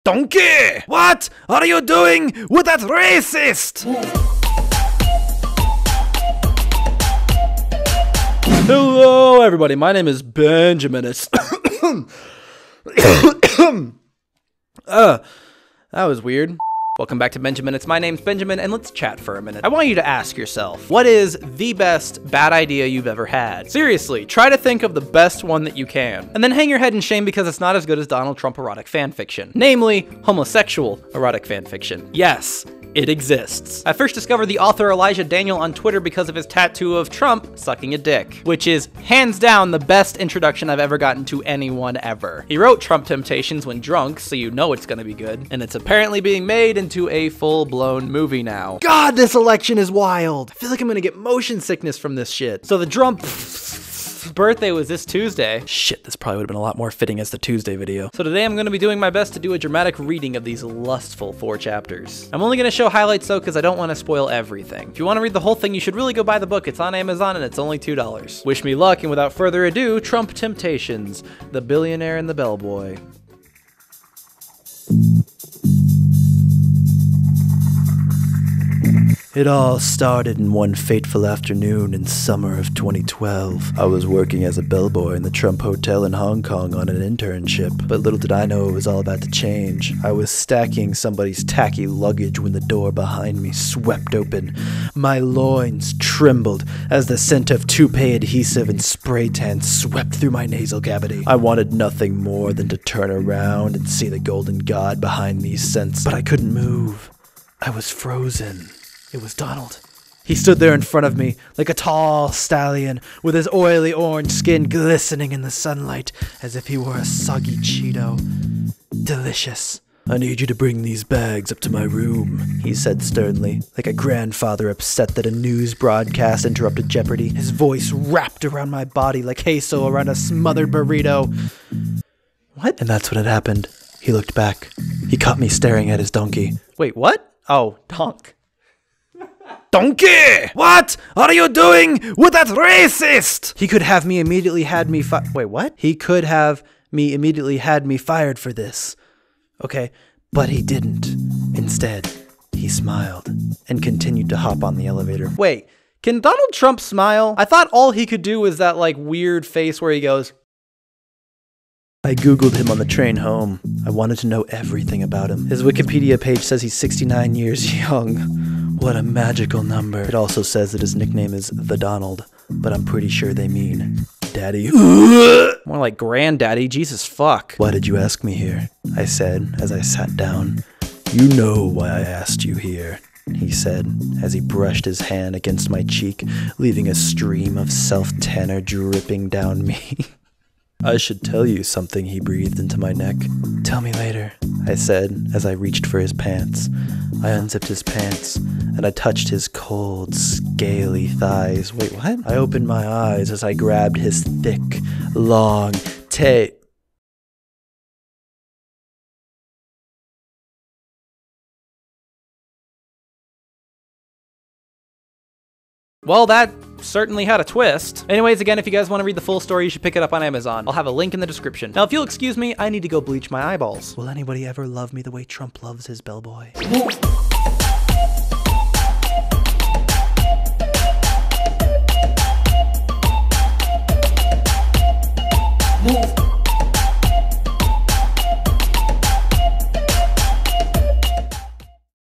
Dramatic Reading: Trump Temptations (NSFW... kinda)
Drumpf sure loves his bell boy.

READ TRUMP TEMPTATIONS FOR YOURSELF!